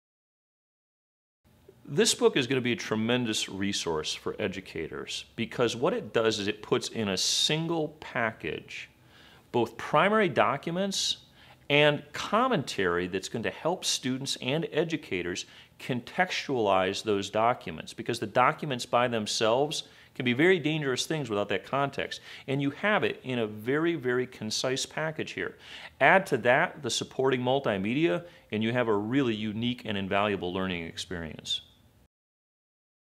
A Unique Learning Experience - Reporting the Revolutionary War (book)
- Military historian John W. Hall explains why Reporting the Revolutionary War (book) is a unique and invaluable learning experience.

John W. Hall is one of 37 contributors to Reporting the Revolutionary War: Before It Was History, It Was News (Sourcebooks, November 2012) by Todd Andrlik. Hall is the Ambrose-Hesseltine Assistant Professor of U.S. Military History at the University of Wisconsin at Madison. He holds a BS in history from the U.S. Military Academy at West Point and a PhD in history from the University of North Carolina--Chapel Hill. He specializes in early American military history with particular emphasis on partisan and Native American warfare. He is the author of Uncommon Defense: Indian Allies in the Black Hawk War (Harvard University Press, 2009) and numerous essays on early American warfare, including "Washington's Irregulars" in A Companion to George Washington, edited by Edward Lengel (Wiley-Blackwell, 2012).